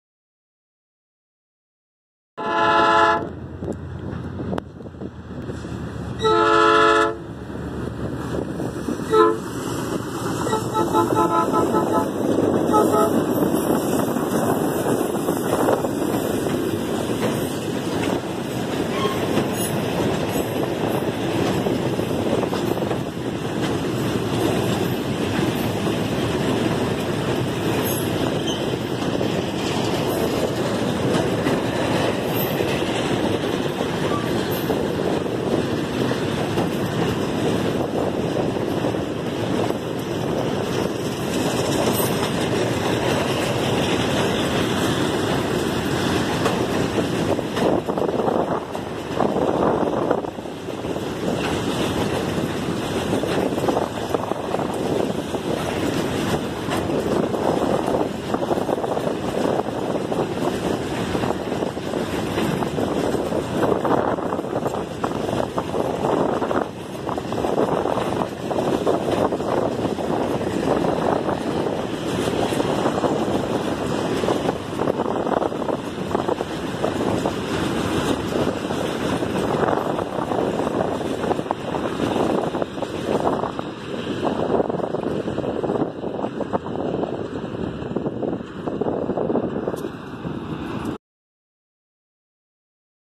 TREM CARGUEIRO, PREFIXO Z27_SÃO GABRIEL/RS
Rumo Logística Multimodal - Trem, Prefixo Z27 Procedente do Porto de Rio Grande/RS passando por São Gabriel/RS indo para o pátio de Cruz Alta/RS